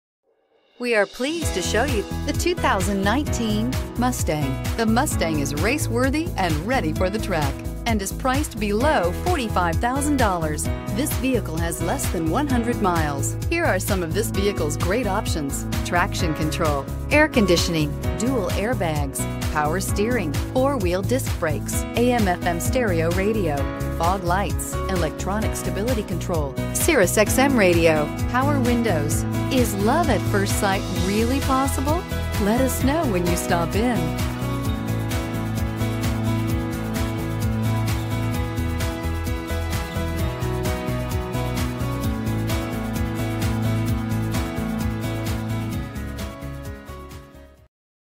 2019 Ford Mustang K5152327
2019 FORD MUSTANG Morrow, GA
Stock# K5152327

Shadow Black 2019 Ford Mustang GT RWD 10-Speed Automatic 5.0L V8 Ti-VCT Mustang GT, 2D Coupe, RWD, Shadow Black, Ebony, 6 Speakers, AM/FM radio, Dual front impact airbags, Dual front side impact airbags, Electronic Stability Control, Exterior Parking Camera Rear, Fully automatic headlights, Outside temperature display, Power door mirrors, Power driver seat, Power passenger seat, Power steering, Power windows, Variably intermittent wipers.

Recent Arrival!Some rebates listed may require customer to finance through Ford Credit, see dealer for details, Price includes: $1,500 - Retail Customer Cash. Exp. 07/01/2019